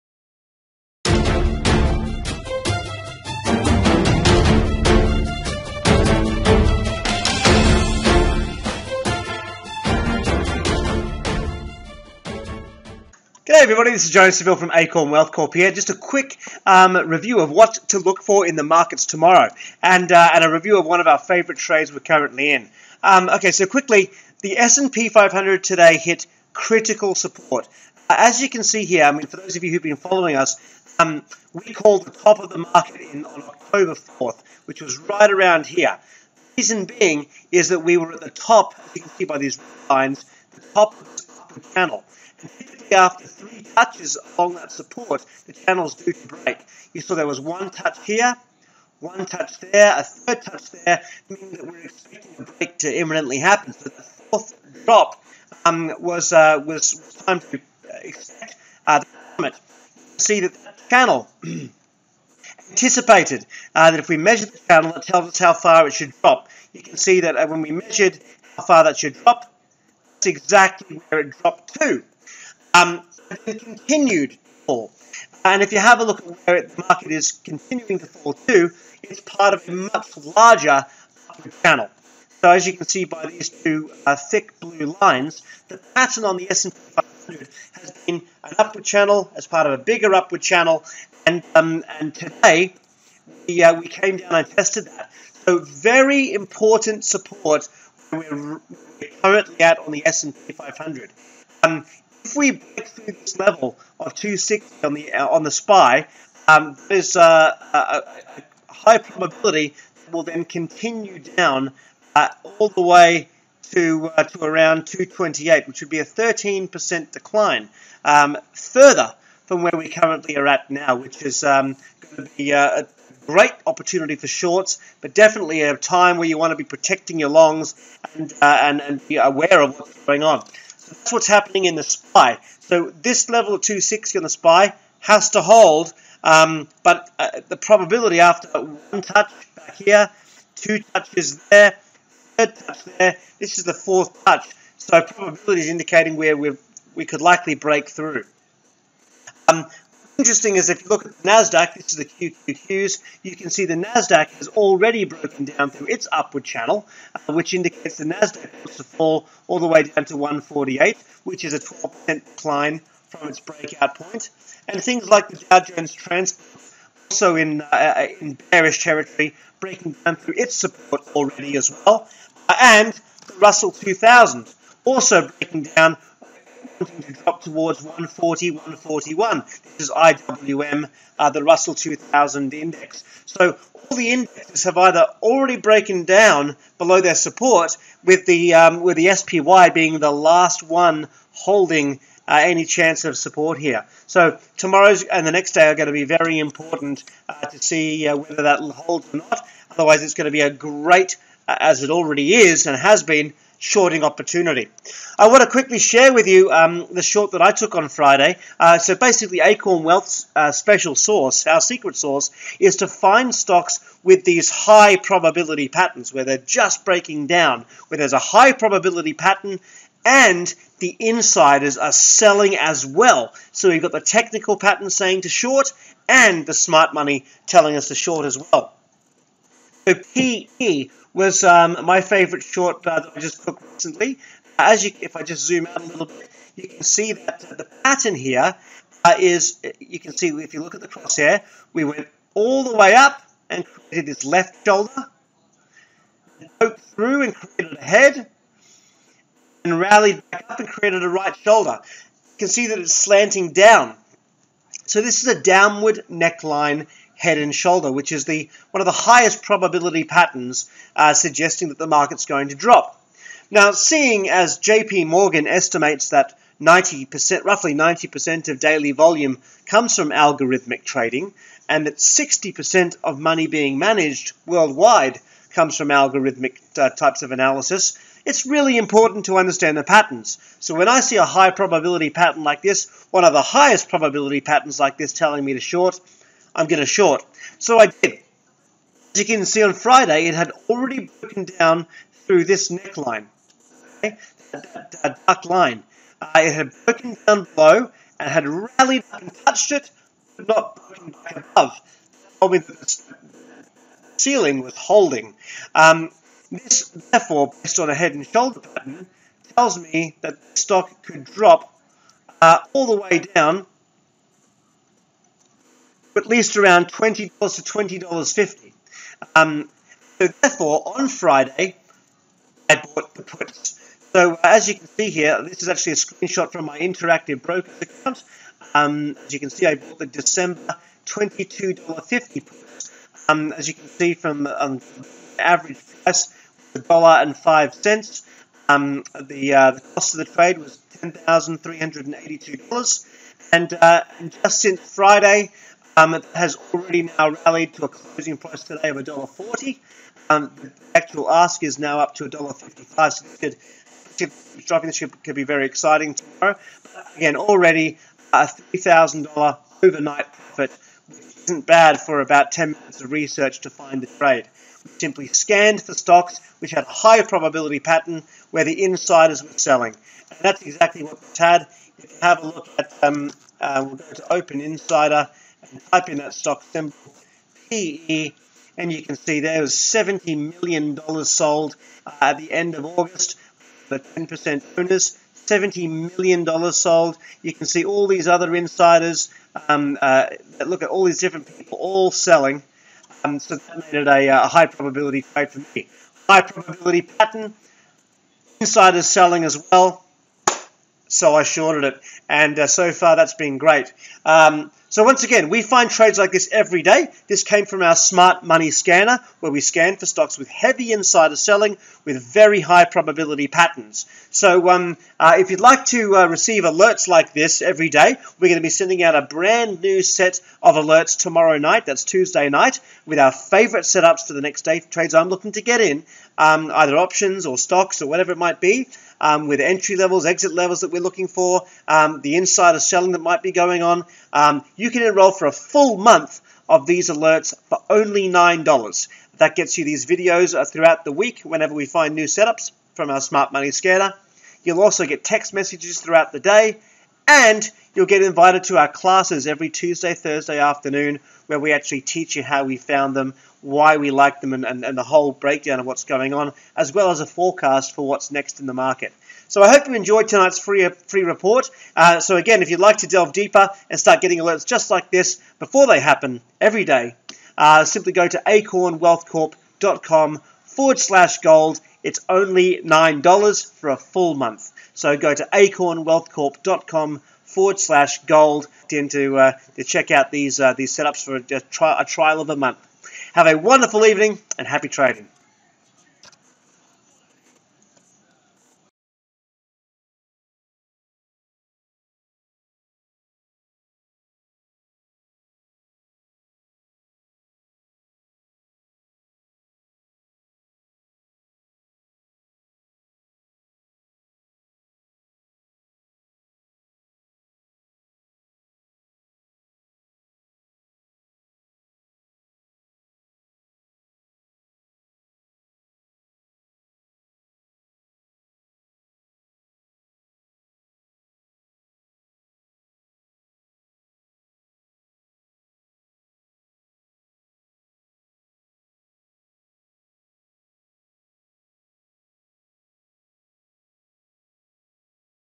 Smart Money Stock Setups for October 30th
Acorn Wealth Review Market Conditions Wednesday on the SPY along with a setup we currently hold on PE.